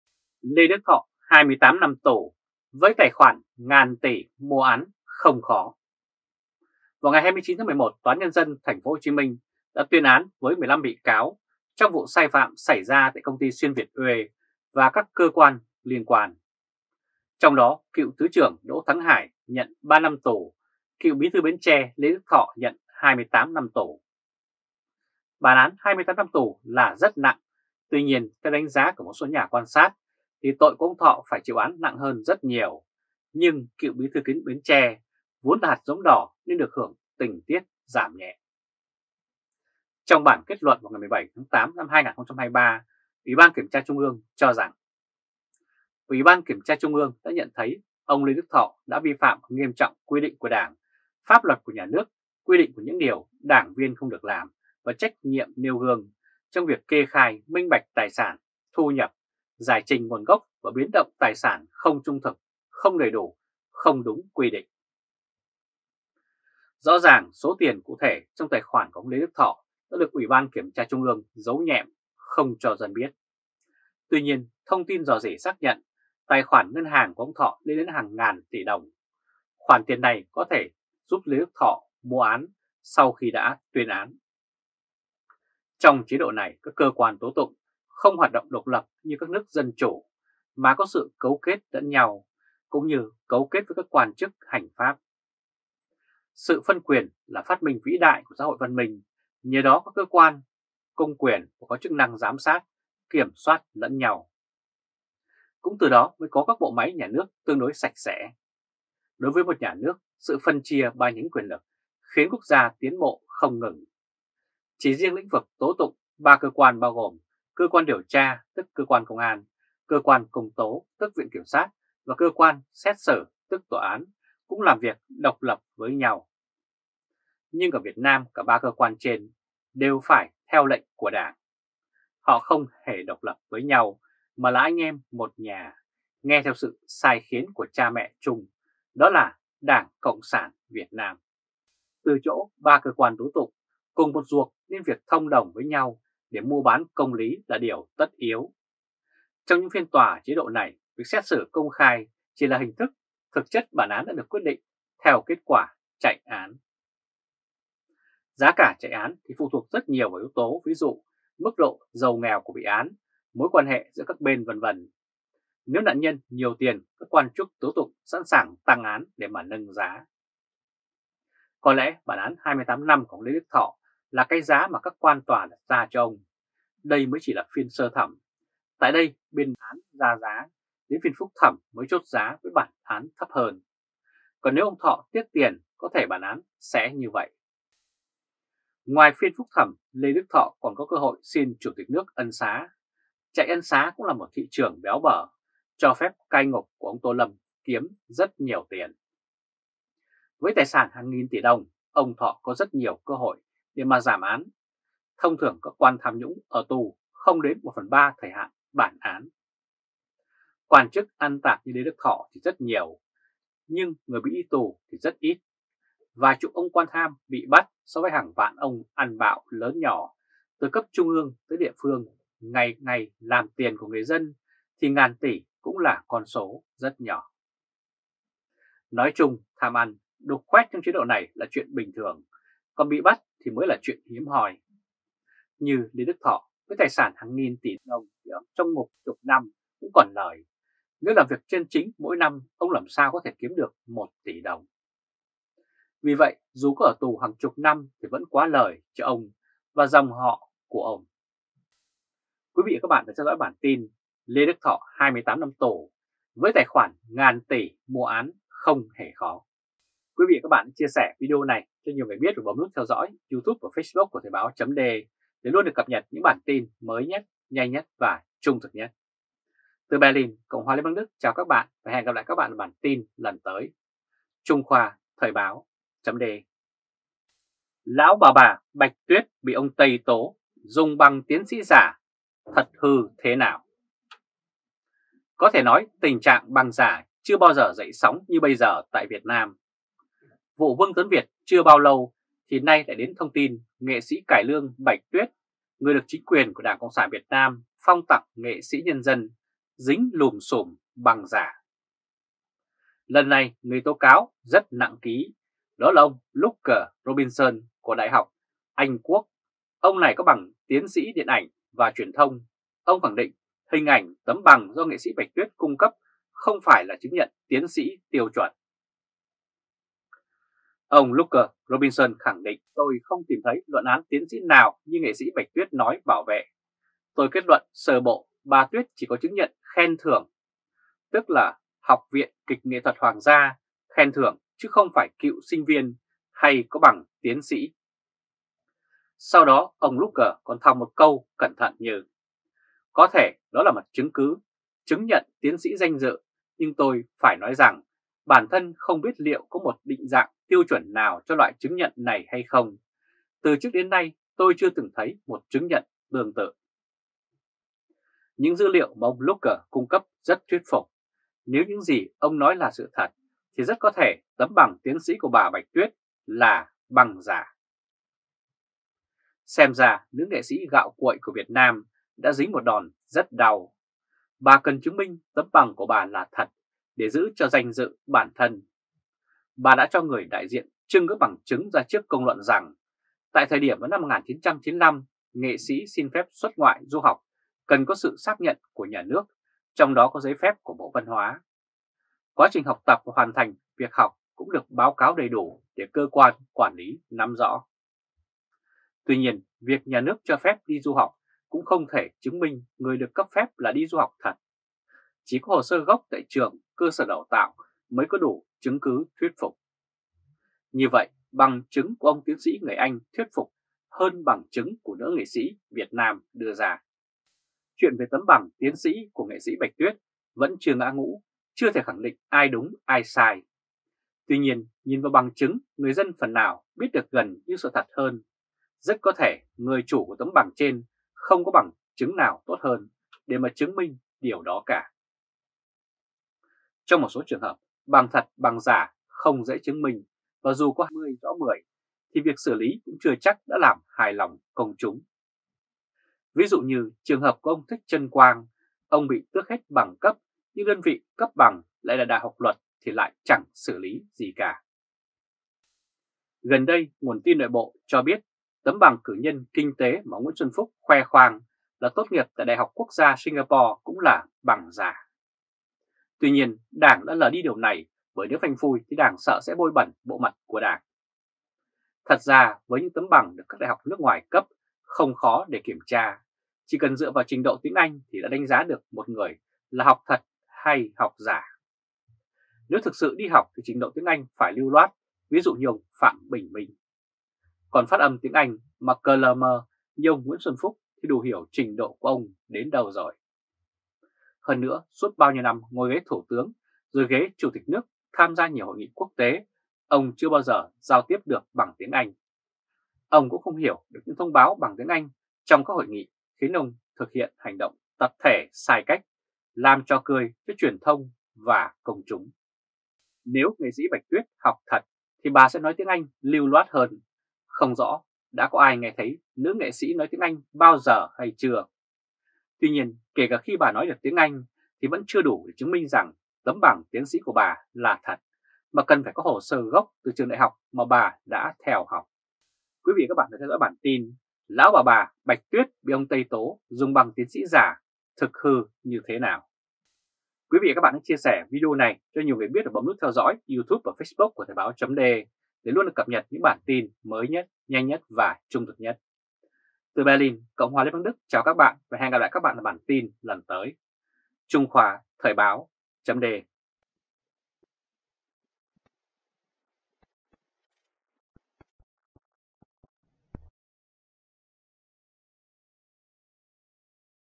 Lê Đức Thọ 28 năm tù, với tài khoản ngàn tỷ mua án không khó!
Lê Đức Thọ - với tài sản nghìn tỷ, thì ở tù chục năm cũng còn lời. Nếu làm việc chân chính, mỗi năm, ông làm sao có thể kiếm được 1 tỷ? Vì vậy, dù có ở tù chục năm, thì vẫn quá lời cho ông và dòng họ.

Xem bài chi tiết tại:
Hoặc: